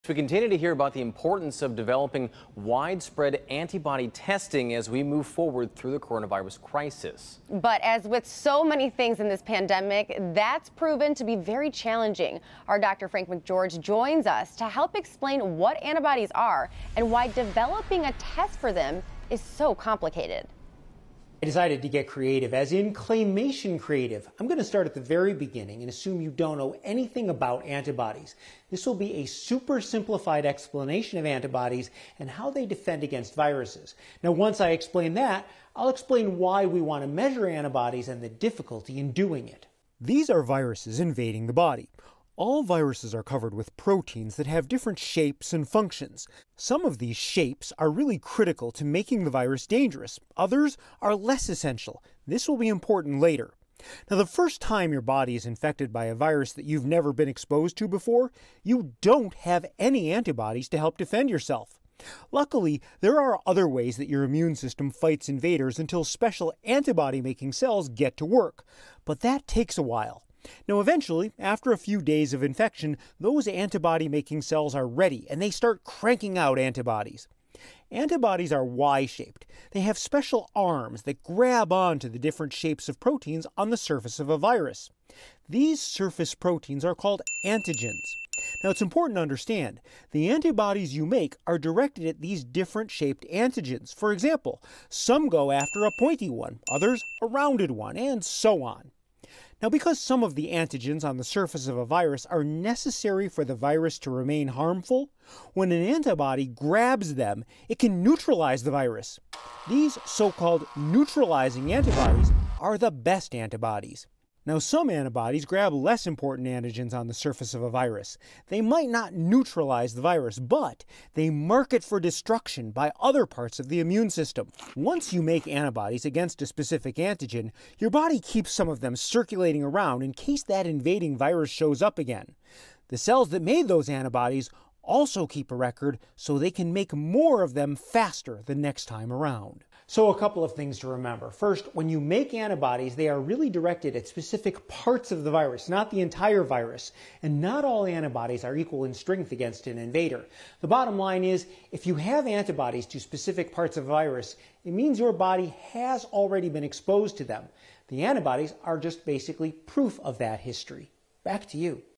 Here's how antibodies defend against viruses in the human body
Local 4's Dr. Frank McGeorge explains what antibodies are and why developing a test for them amid the coronavirus pandemic is so complicated.